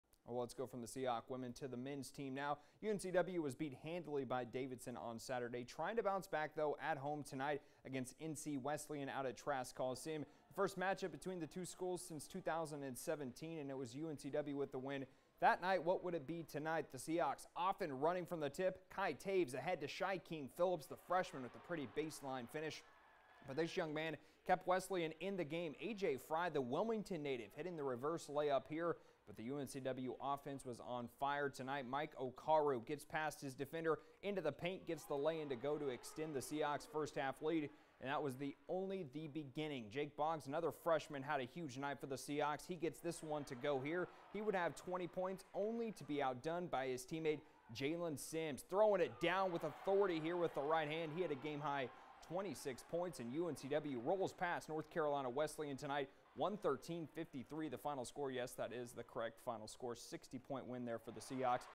UNCW Men's Basketball rolls over N.C. Wesleyan at home
WILMINGTON,NC (UNCW Sports)--The UNCW men’s basketball team connected on 11 of its season-high 17 three-pointers in the second half en route to a school-best 60-point victory margin in a 113-63 manhandling of Division III N.C. Wesleyan Tuesday night at Trask Coliseum.